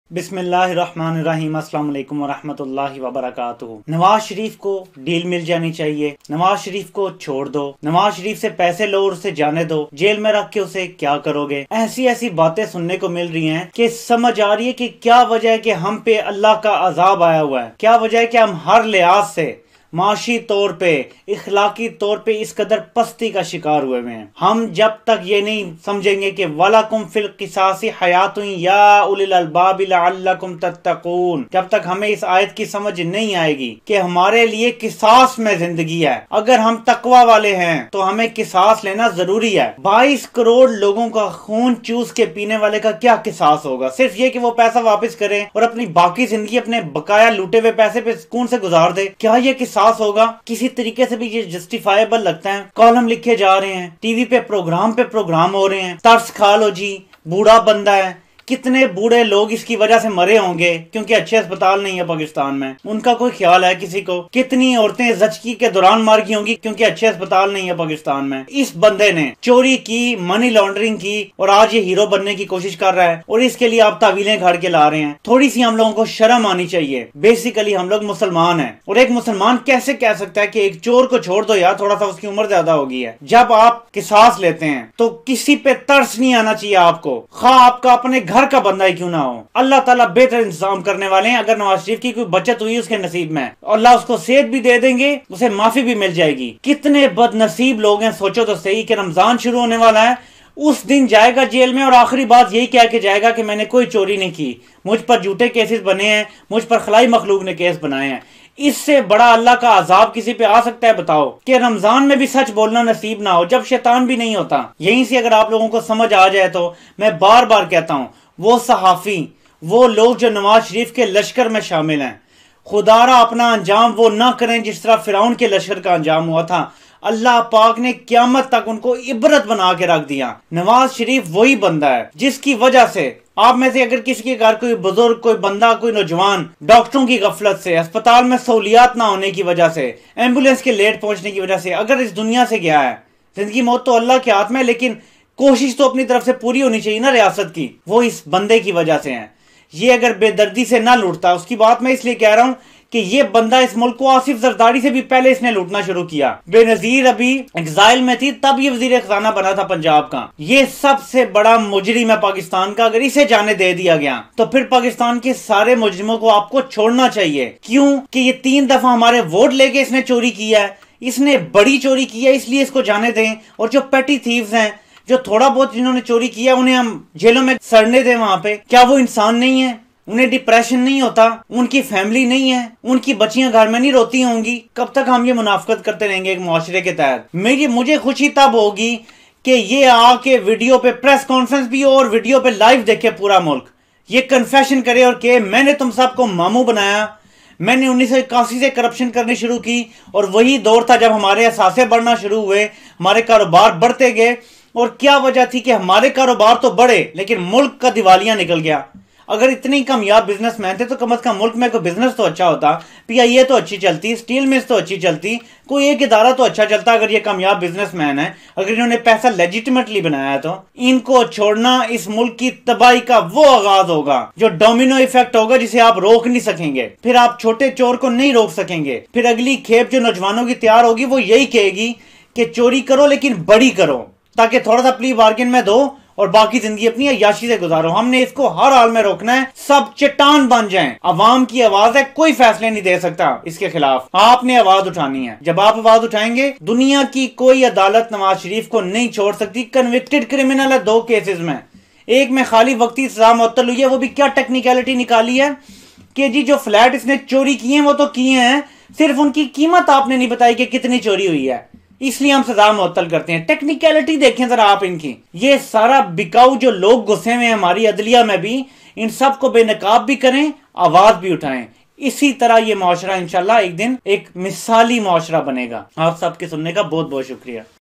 **NRO To Nawaz Would Be Catastrophic** For Pakistan Must Raise Our Voices
Why We Must Stand Up And Say No To Any Kind Of NRO Given To Nawaz Sharif And Why It Is Imperative To Keep Him In Pakistan.